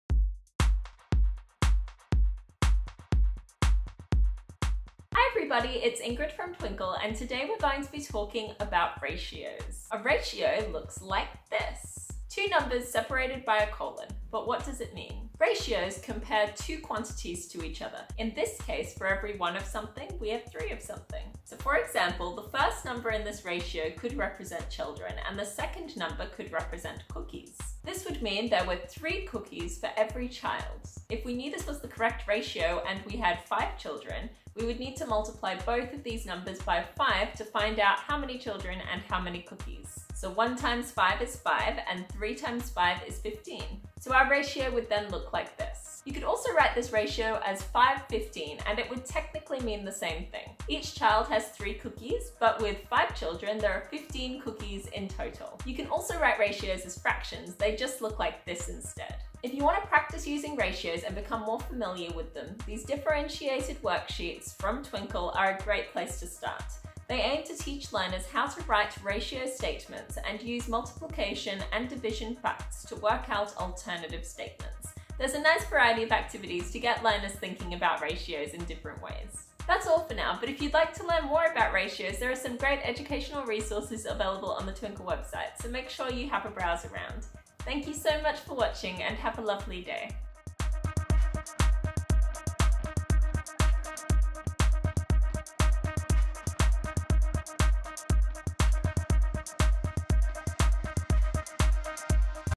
What Are Ratios in Math?
We hope you enjoyed learning about what a ratio is.

So, what are ratios in math? Ratios compare two quantities to each other. In this case for every one of something, there are 3 of something.

The Twinkl team upload videos regularly Monday to Friday with tips for teachers, homework help guides and home learning support for parents.